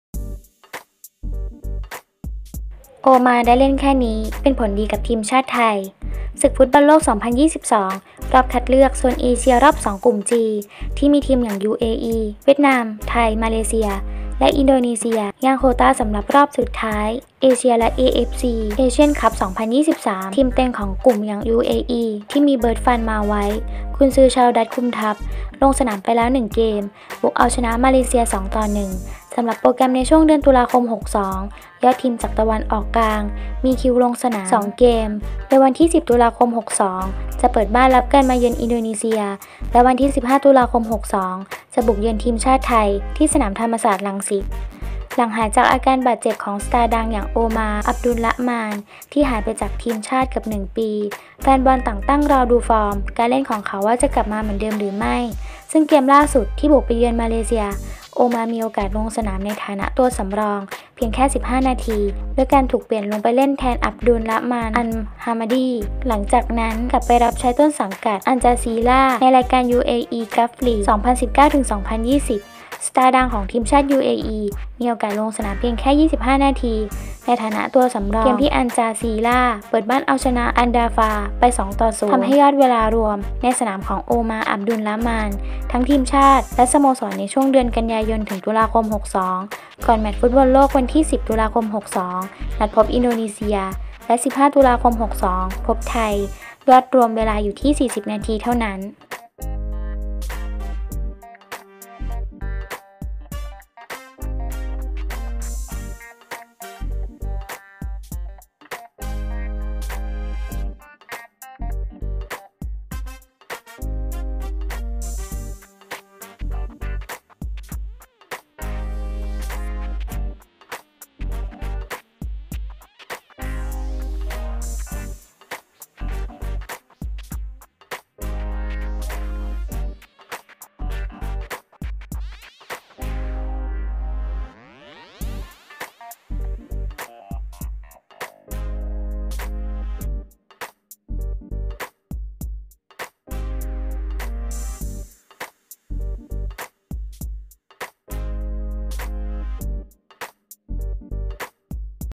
โอมาร์ ได้เล่นแค่นี้ เป็นผลดีกับทีมชาติไทย
ศึกฟุตบอลโลก 2022 รอบคัดเลือกโซนเอเชีย รอบสอง กลุ่ม จี ที่มีทีมอย่าง ยูเออี,เวียดนาม,ไทย,มาเลเซีย และ อินโดนีเซีย แย่งโควตาสำหรับรอบสุดท้ายเอเชียและเอเอฟซี เอเซียน คัพ 2023 ทีมเต็งของกลุ่มอย่าง ยูเออี ที่มี เบิร์ต ฟาน มาไวค์ กุนซือชาวดัตซ์ คุมทัพ ลงสนามไปแล้วหนึ่งเกม บุกเอาชนะ มาเลเซีย 2-1 สำหรับโปรแกรมในช่วงเดือน ต.ค.62 ยอดทีมจากตะวันออกกลางมีคิวลงสนาม 2 เกม วันที่ 10 ต.ค.62 เปิดบ้าน รับการมาเยือน อินโดนีเซีย และ วันที่ 15 ต.ค.62 บุกเยือนทีมชาติไทย ที่สนามธรรมศาสตร์ รังสิต